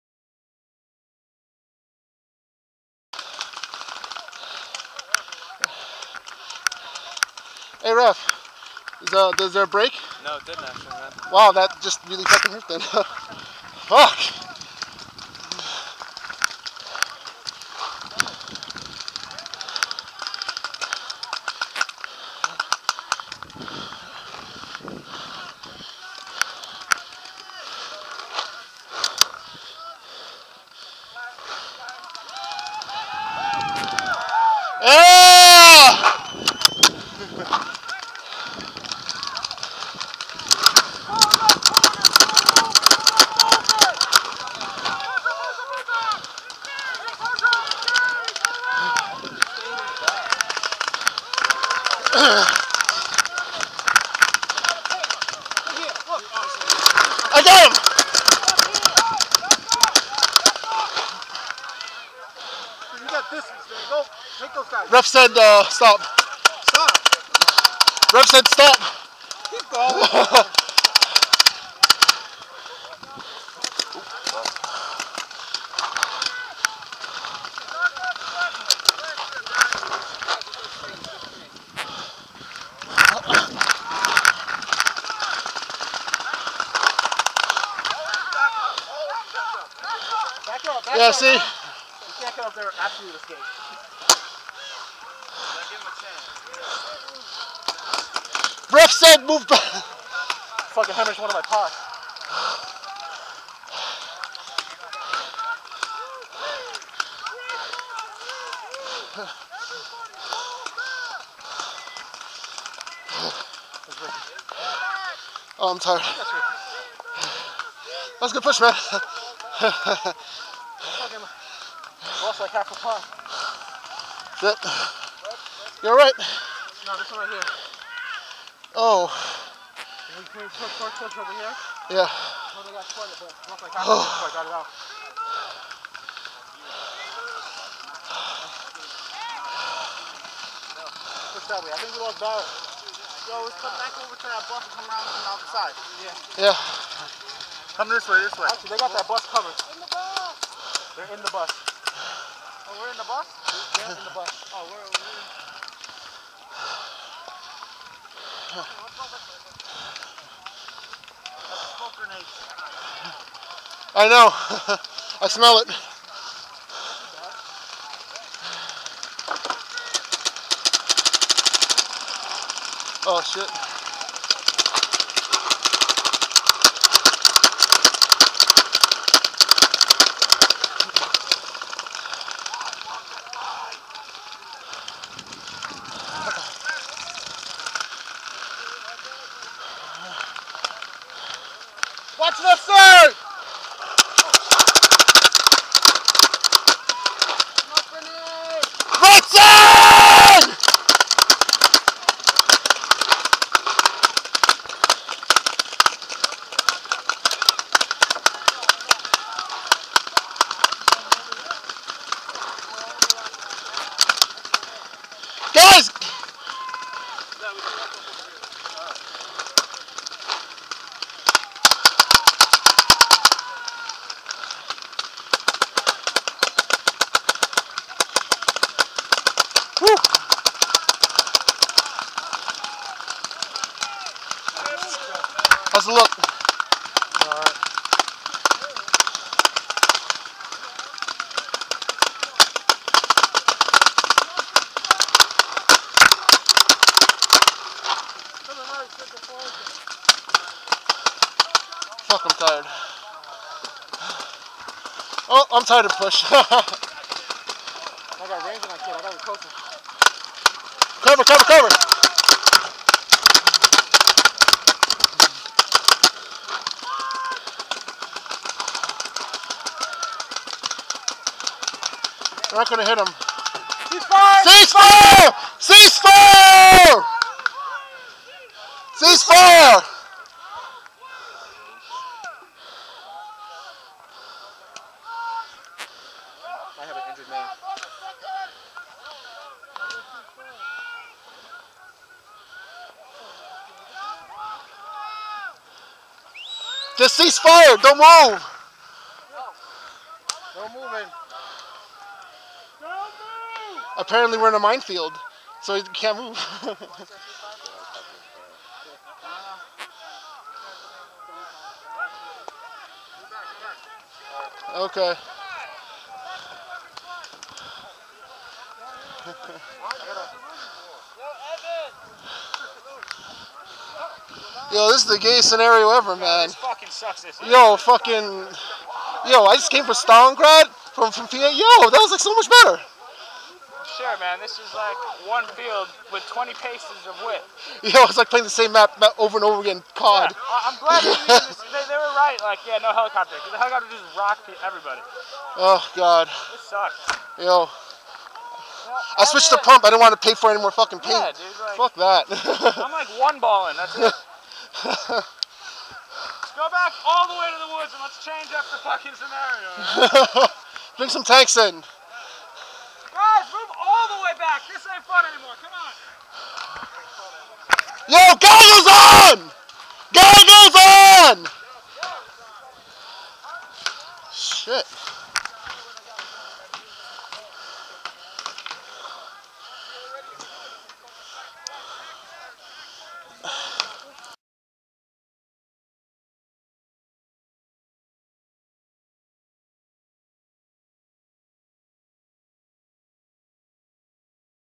Cousins Paintball Big Game Brawl 2012 Blue Hobo Ninja Back into Action 5/19/2012 Autococker Pump
We were Blue
Hobo Ninja goes to a Ref to do a paint check before flying back into action!
Grumpy Pumpie Debuts
Cousin's Paintball Big Game at Riverhead Long Island
This event was originally supposed to be the Big Game Scenario at Coram but was changed due to an Eviction noticed served to Cousins in Coram Location.
This Video is partially grey scale due to color distortion experienced with the file.
We did not want to lose footage due to this problem and posted it in grey scale so that it would be viewable.